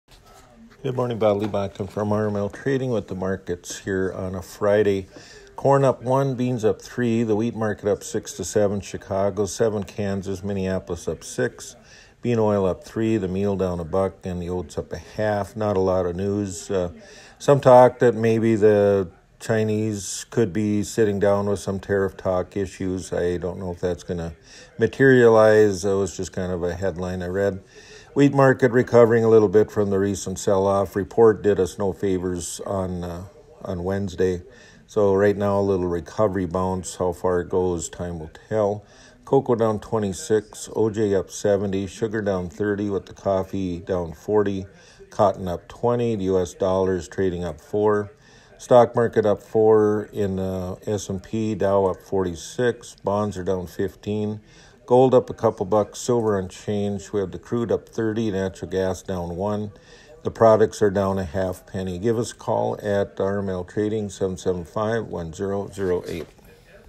Friday Ag Market Report
GRAND FORKS, ND (iNewZ.TV) Your Friday ag market report from RML Trading in Grand Forks.